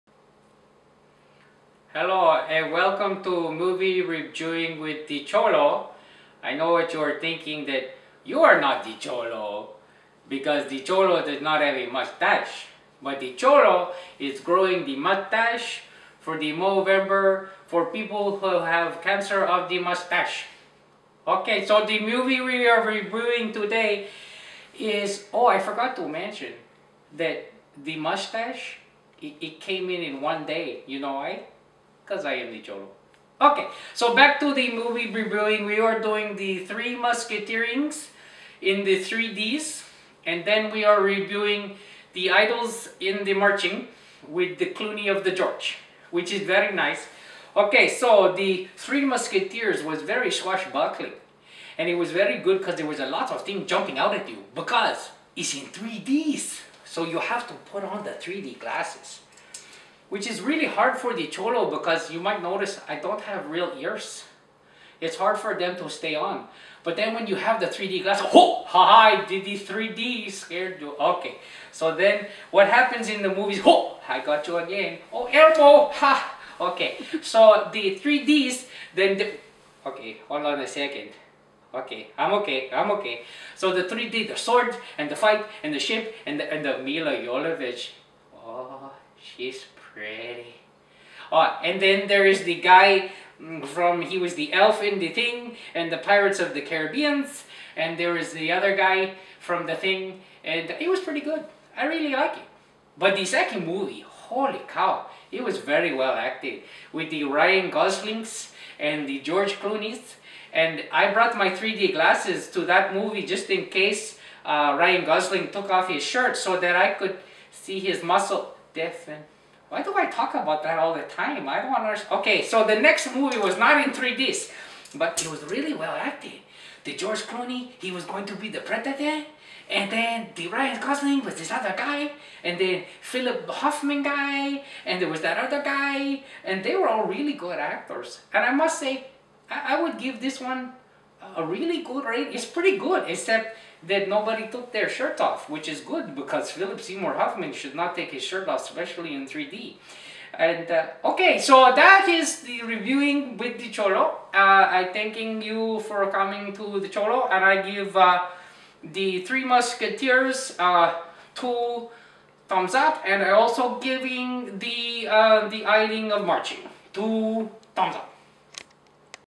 3 Musketeers & The Ides of March The Cholo Reviews.MOV
The Cholo Reviews - The Three Musketeers, Orlando Bloom, Luke Evans, Milla Jovovich, Ray Stevenson: The Ides of March, George Clooney, Ryan Gosling, Philip Seymour Hoffman